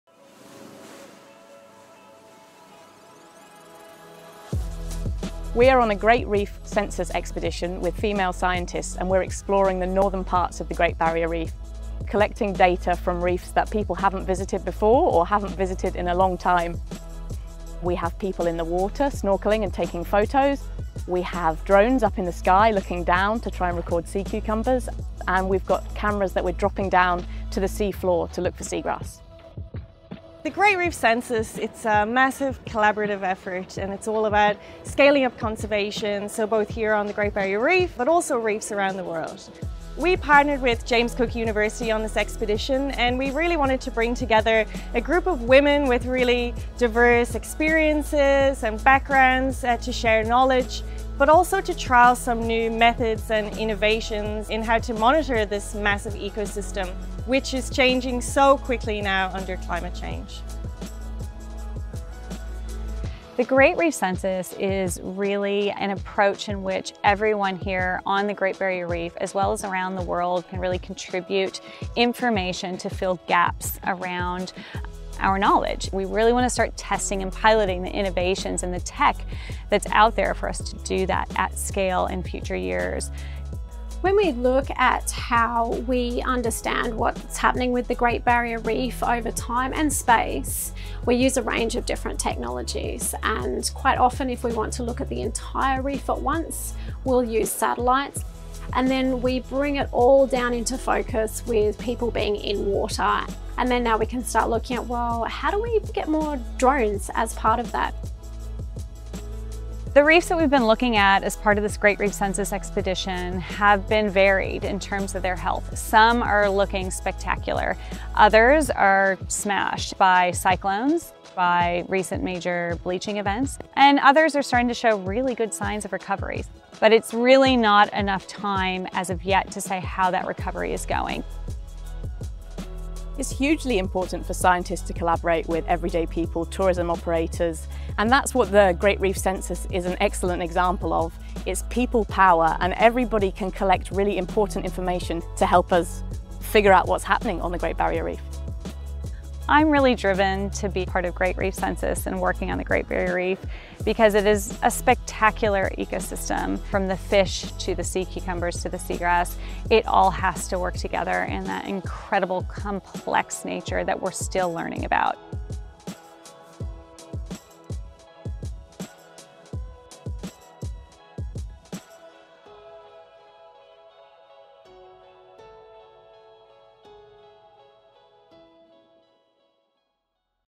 Reef Women expedition, Great Reef Census 2021
In late October 2021, 15 women from a diverse range of backgrounds came together as part of a Great Reef Census expedition, surveying 46 reefs across the Ribbon Reefs and north of Lizard Island!

Bringing together science, conservation, tourism and people-power, our Reef Women expedition was a great example of all we can achieve through collaboration 🙌 Whether you're a scientist, a skipper or an everyday person, we all have a role to play to protect this incredible ecosystem.

🎥 Grumpy Turtle Creative